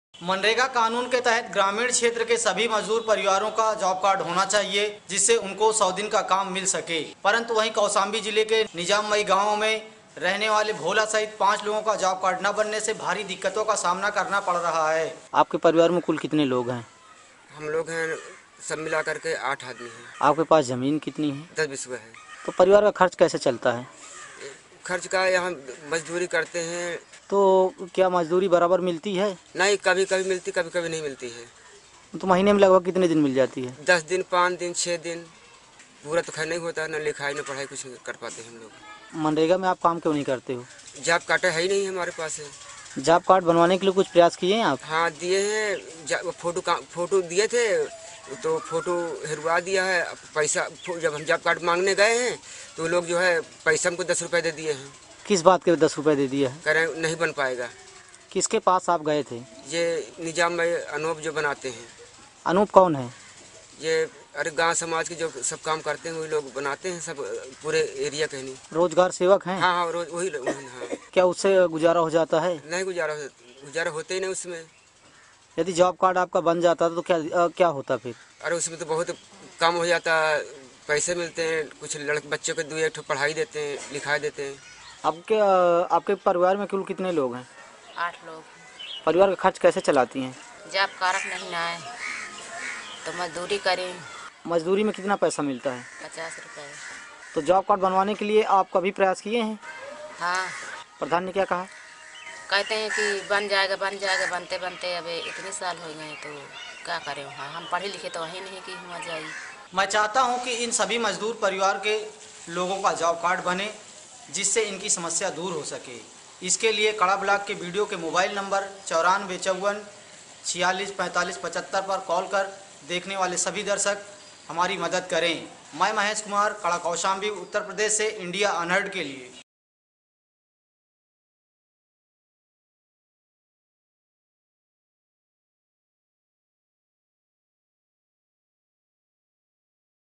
No Job Card in Nizammai, Uttar Pradesh— Video Volunteer Mahesh reports
This video was made by a Video Volunteers Community Correspondent Mahesh Kumar.